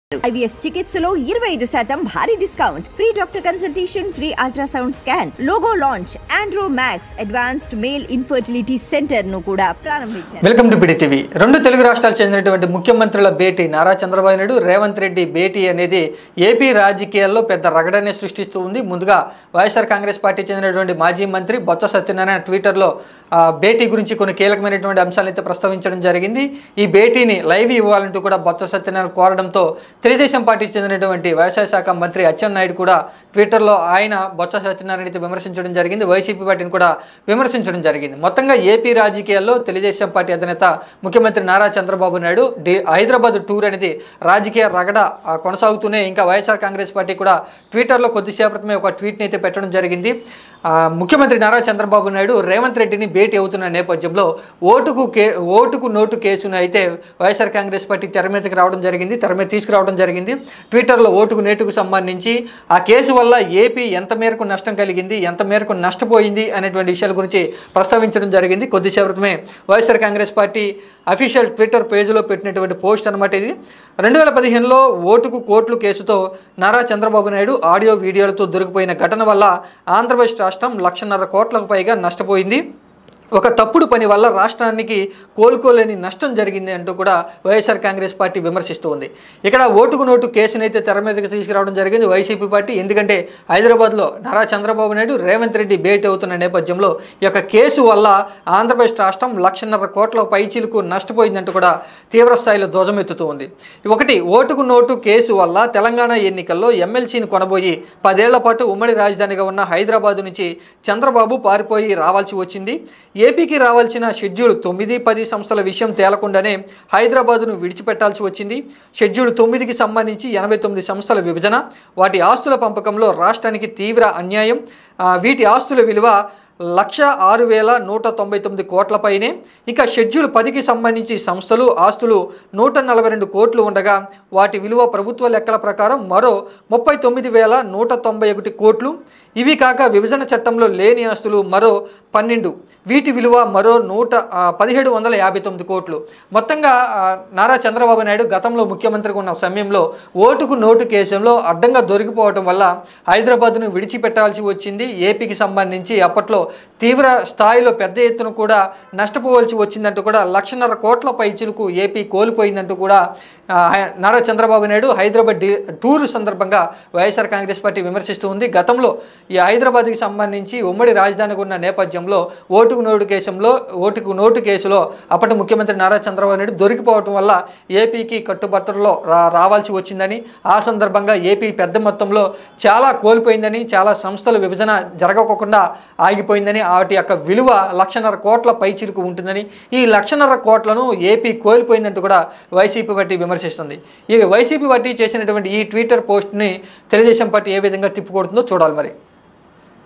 YSRCP Sensational Tweet on Vote to Note Case About CM Chandrababu : PDTV News
YSRCP Sensational Tweet on Vote to Note Case About CM Chandrababu

Watch the latest news on YSRCP's sensational tweet about the Vote to Note case involving CM Chandrababu. Stay updated with PDTV for all the latest updates on YS Jagan and CBN.